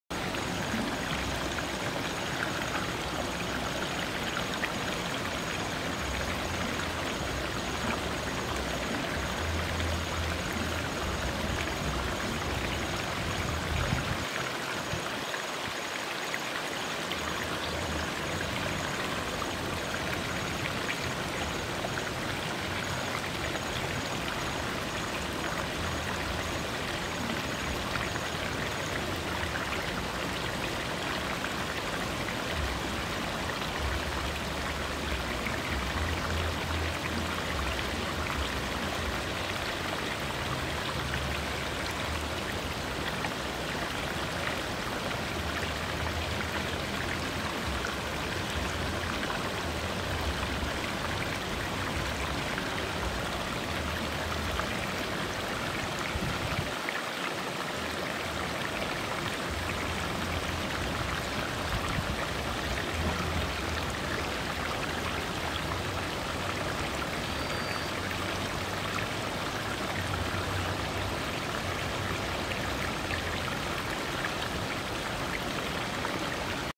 (background video) air yang mengalir di kebun padi,
suasana desa yang indah, air yang mengalir di perkebunan padi.